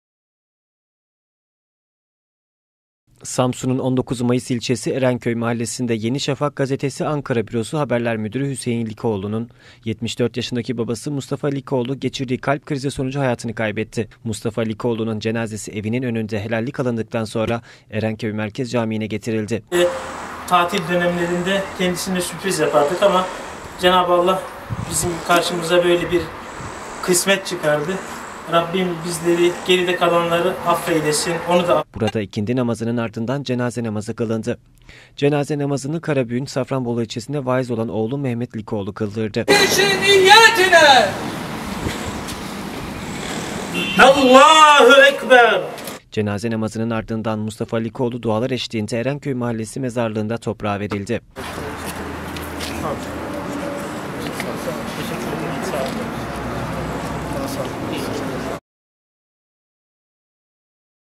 LİKOĞLU'NUN ACI GÜNÜ
Samsunlu gazeteci Hüseyin Likoğlu'nun babası Mustafa Likoğlu geçirdiği kalp krizi sonucu hayatını kaybetti. Cenaze namazını vaiz oğlunun kıldırdığı Likoğlu'nun cenazesi dular eşliğinde son yolculuğuna uğurlandı.